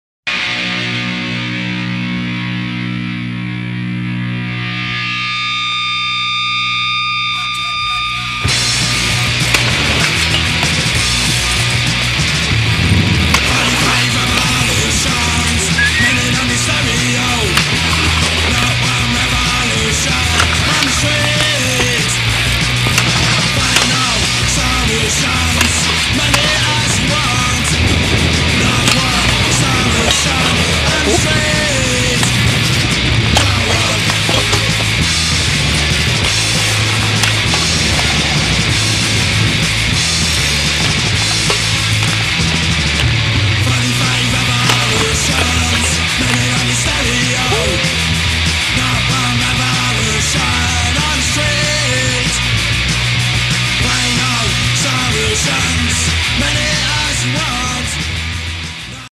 [Lifestyle BMX] Skoone Bastion rail session with Zhenya.
Quick flat rail session at the local d.i.y spot.